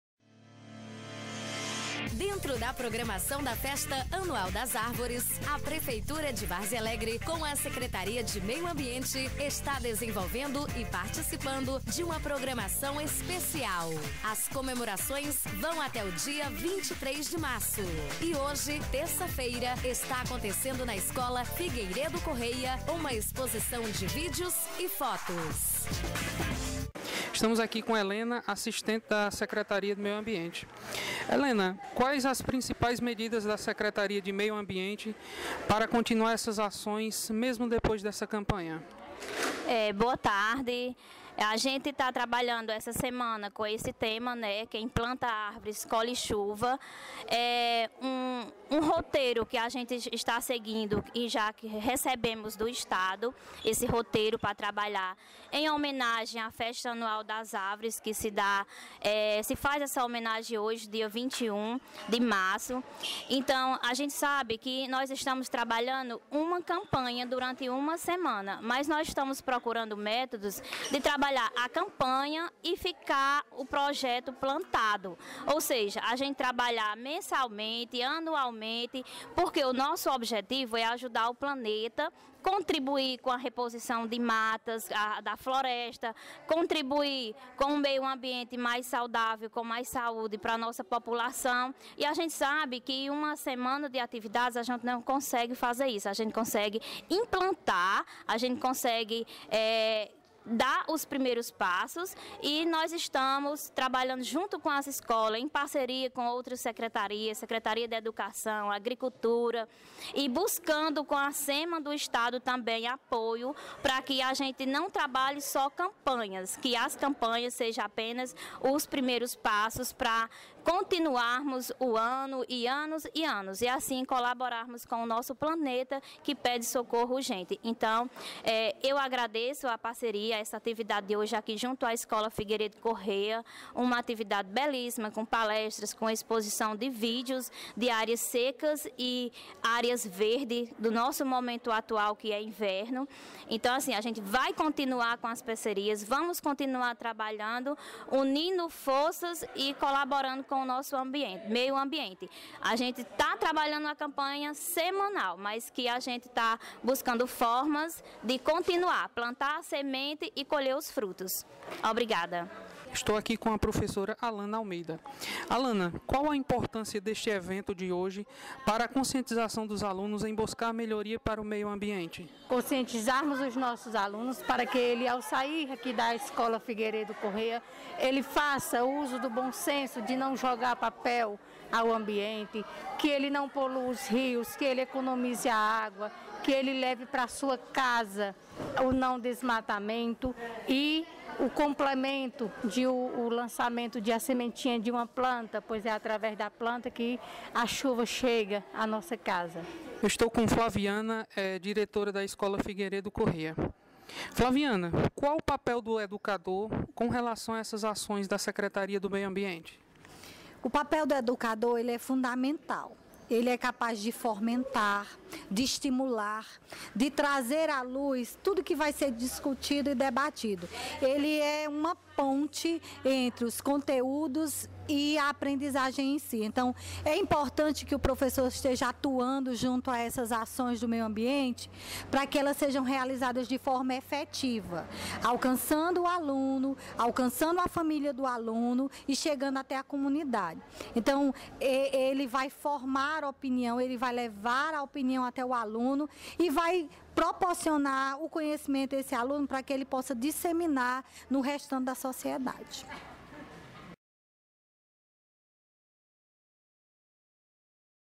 Exposição de fotos e vídeos sobre o meio ambiente
Dentro da programação da Festa Anual das árvores, a prefeitura de Várzea Alegre, com a Secretaria de Meio Ambiente está desenvolvendo e participando de uma programação especial. As comemorações vão até o dia 23 de março e hoje, terça-feira, está acontecendo na escola Figueiredo Correia, uma exposição de vídeos e fotos.